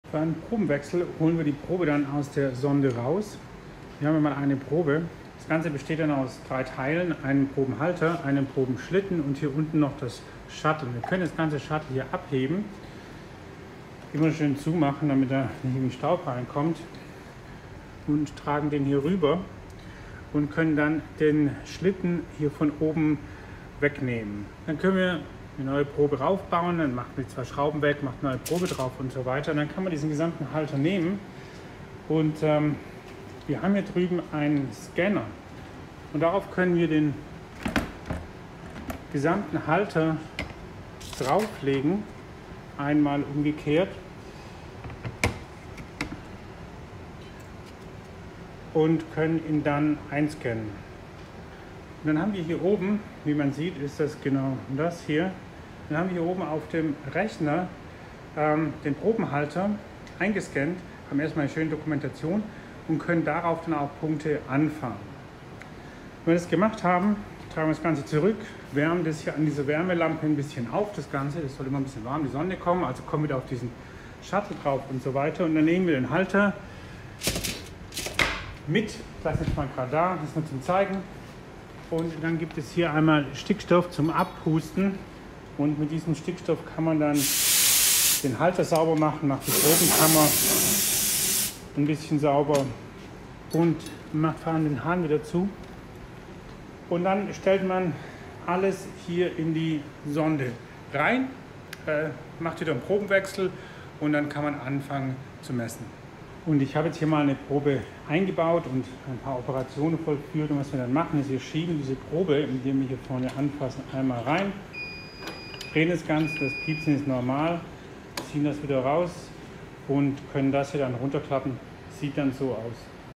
Einblick in den Mess-Alltag mit EPMA/SEM: Einbau einer Probe in die EPMA
Hier nur eine Kurzversion, wie eine Probe eingebaut wird, um die Idee davon zu vermitteln. Auf geoplatform.de gibt es mehr Videos dazu.
Schaue Dir alles Kursmaterial und noch viel mehr an auf geoplatform.de.